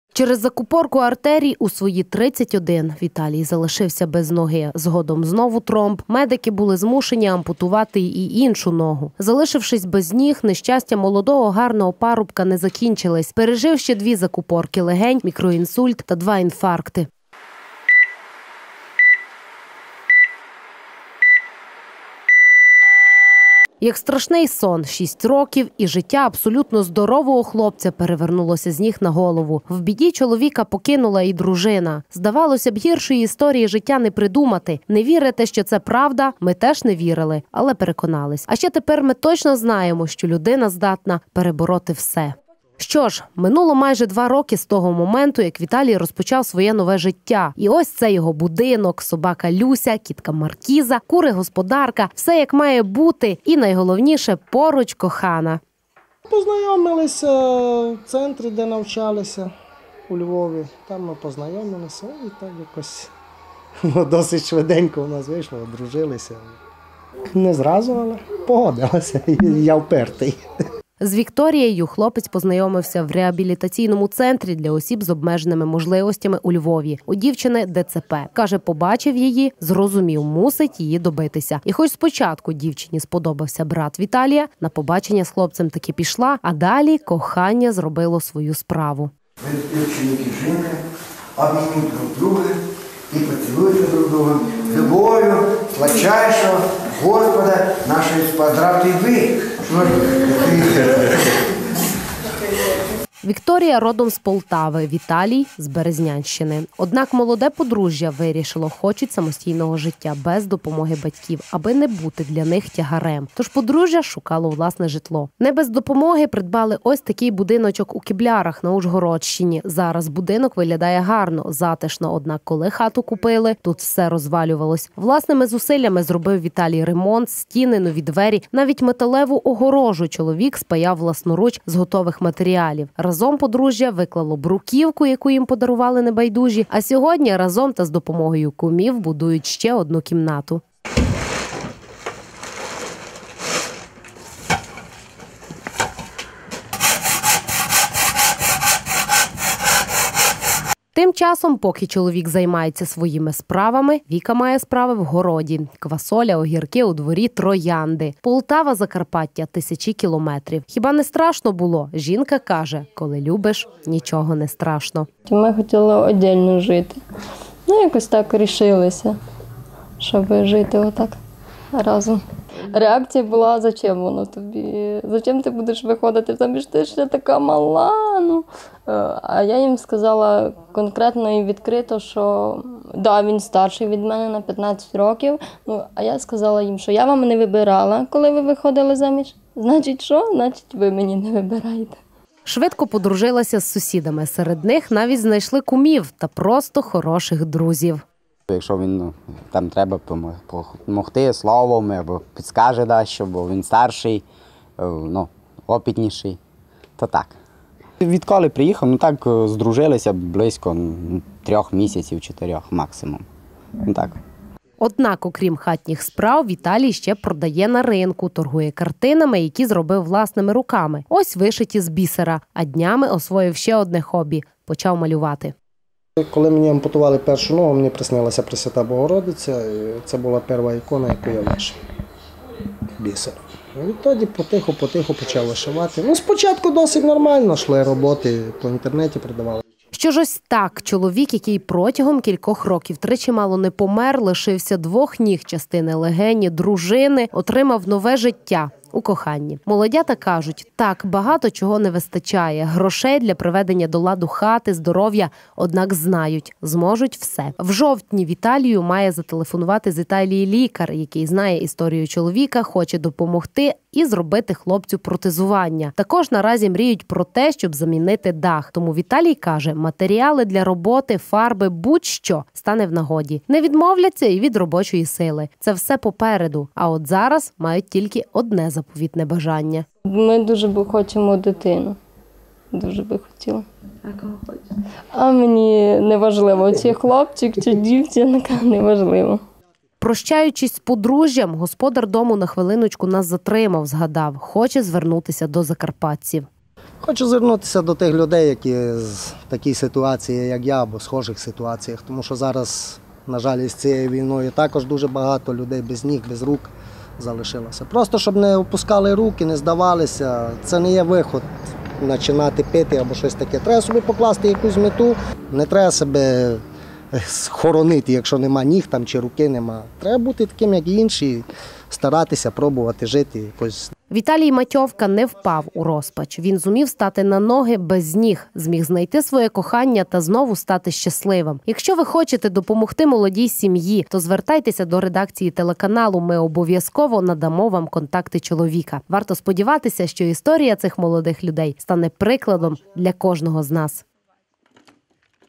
Чоловік без ніг зумів розпочати життя спочатку
Через закупорку артерій у свої 31 Віталій залишився без ноги, згодом знову тромб, медики були змушені ампутувати і іншу ногу. Залишившись без ніг, нещастя молодого, гарного парубка не закінчились: пережив ще двічі закупорку легень, мікро інсульт та два інфаркти. Шість років і життя абсолютно здорового хлопця перевернулося з ніг на голову, чоловіка покинула і дружина.. Минуло майже два роки з того моменту, як Віталій розпочав своє нове життя.